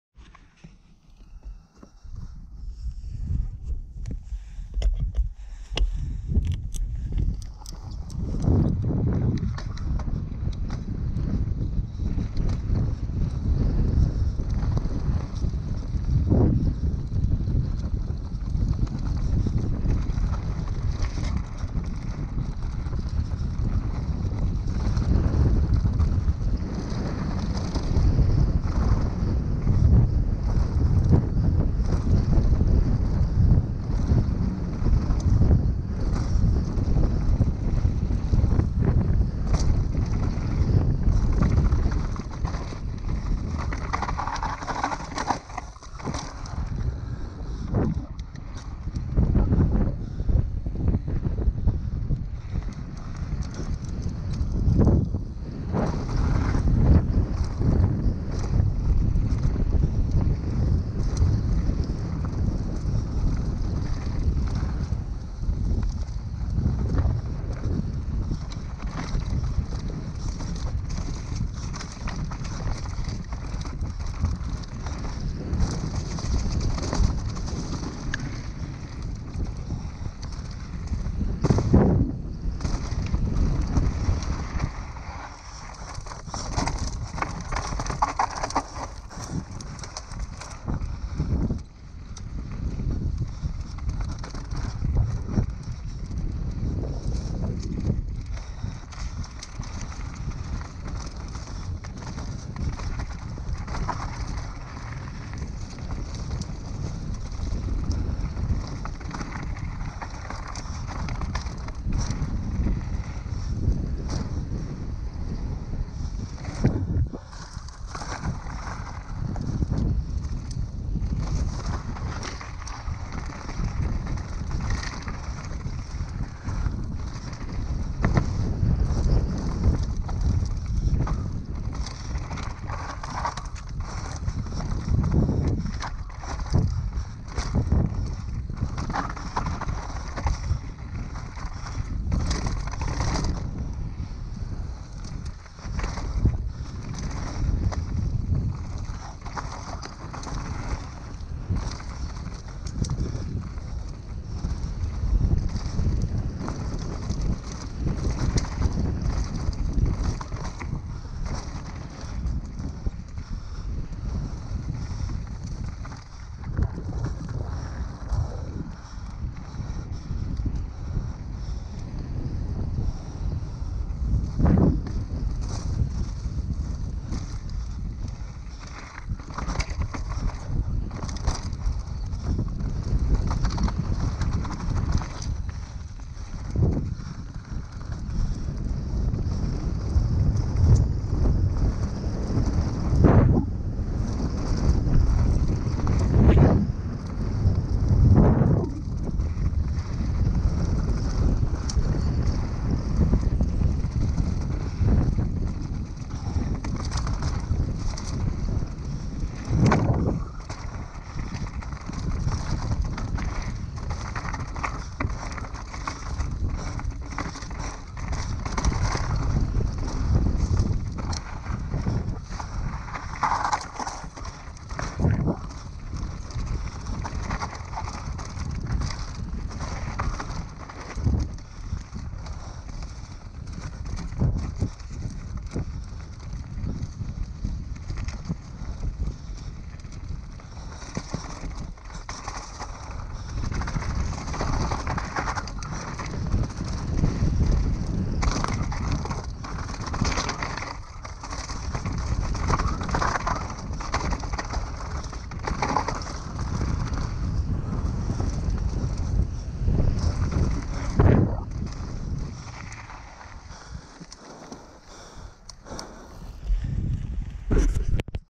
Les Débalaïres GOPRO FULL RUN
Back in the game avec une vidéo qui va faire 200 vues pcq y'a pas de roues arrière.
Bike : Saracen Myst Team 29 2019
Gopro HERO 6 black
(tourner avant le confinement, restez chez vous bordel)
Si y'a d'autre questions posés les moi en commentaire, c'est faire pour ca ;)